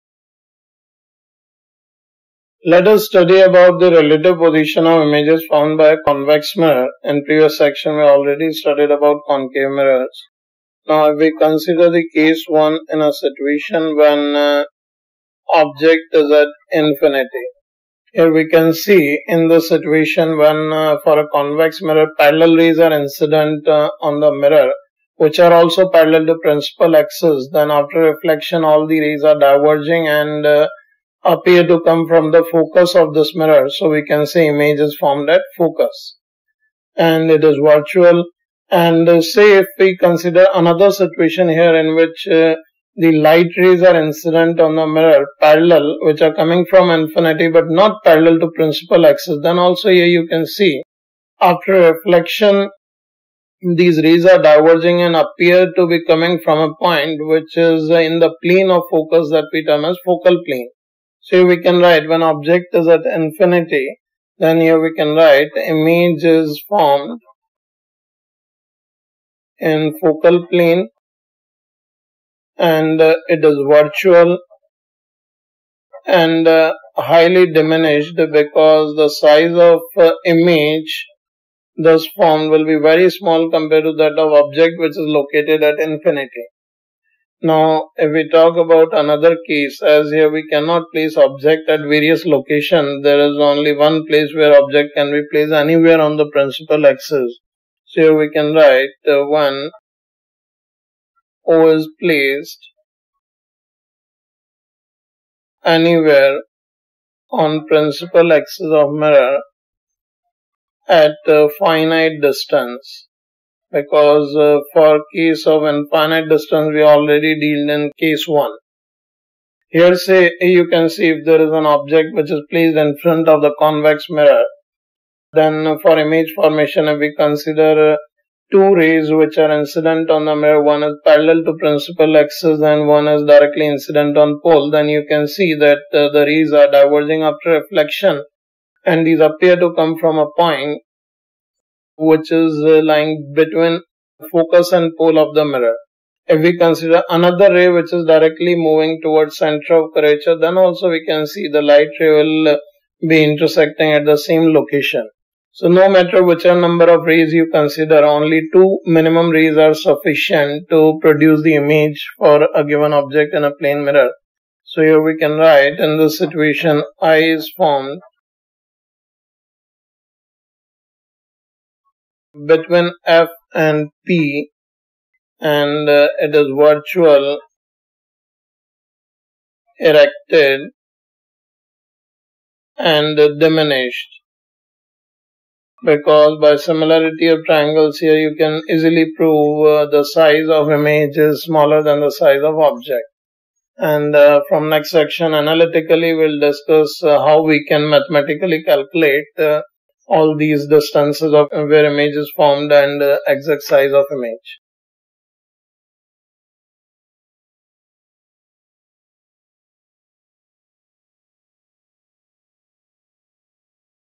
Class 12 Physics | Reflection of Light | #29 Relative Position of Image Formed by a Convex Mirror
PG Concept Video | Reflection of Light | Relative Position of Image Formed by a Convex Mirror by Ashish Arora

Students can watch all concept videos of class 12 Reflection of Light for jee & neet in proper sequence through PG channel playlist.